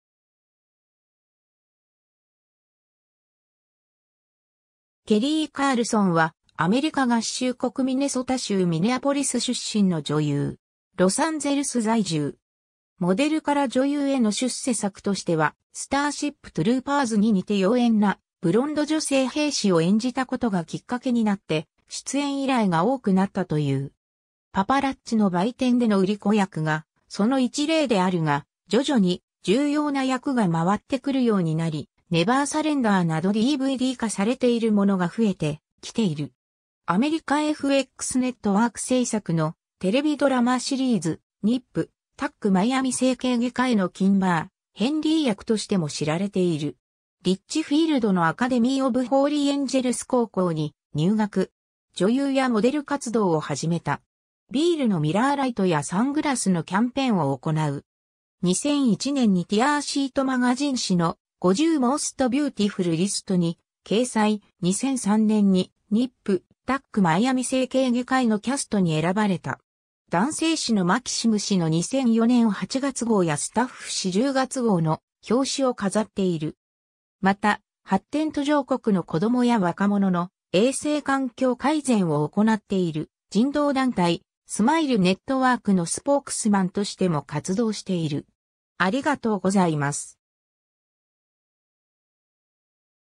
ケリー・カールソン
ケリー・カールソン（Kelly Carlson、1976年2月17日 - ）は、アメリカ合衆国ミネソタ州ミネアポリス出身の女優。
ロサンゼルス在住。
 モデルから女優への出世作としては『スターシップ・トゥルーパーズ2』にて妖艶なブロンド女性兵士を演じた事がきっかけ（DVDの監督コメンタリーによると脱ぎっぷりがいいらしい）になって出演依頼が多くなったという。
『パパラッチ』の売店での売り子役がその一例であるが、徐々に重要な役が回ってくるようになり、『ネバー・サレンダー』などDVD化されているものが増えてきている。
 アメリカ・FXネットワーク制作のテレビドラマシリーズ『NIP/TUCK マイアミ整形外科医』のキンバー・ヘンリー役としても知られている。
 リッチフィールドのAcademy of Holy Angels高校に入学。
女優やモデル活動を始めた。
ビールのミラー・ライトやサングラスのキャンペーンを行う。
2001年にTear Sheet Magazine誌の「50 Most Beautiful list」に掲載、2003年に『NIP/TUCK マイアミ整形外科医』のキャストに選ばれた。
 男性誌のMaxim誌の2004年8月号やStuff誌10月号の表紙を飾っている。
 また、発展途上国の子供や若者の衛生環境改善を行っている人道団体、スマイル・ネットワークのスポークスマンとしても活動している。